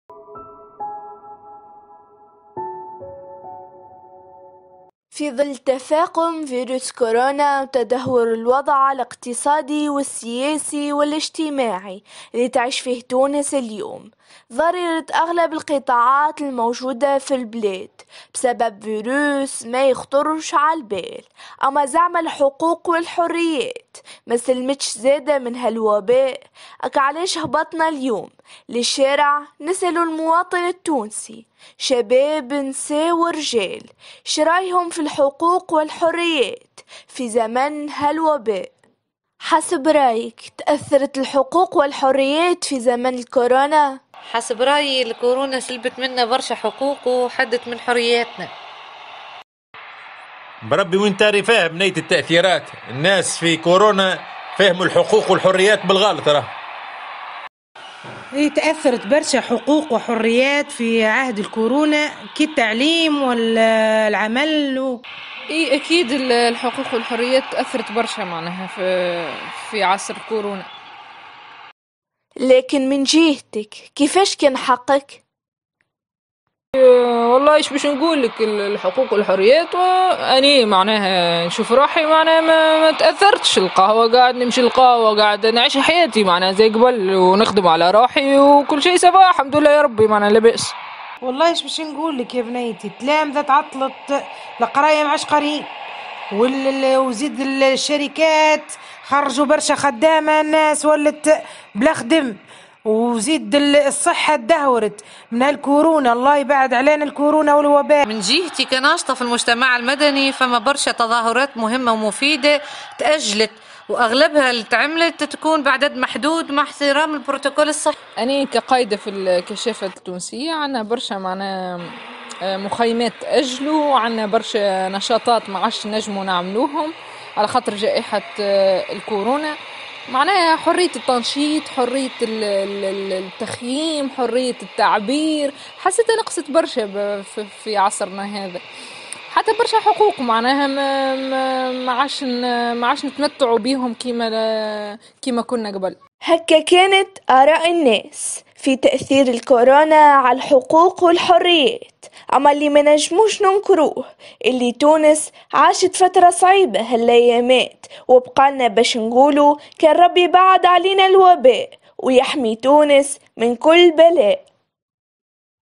تأثير الكورونا على حقوقنا وحرياتنا
عمل لمريم الشريف
راديو بالما
قابس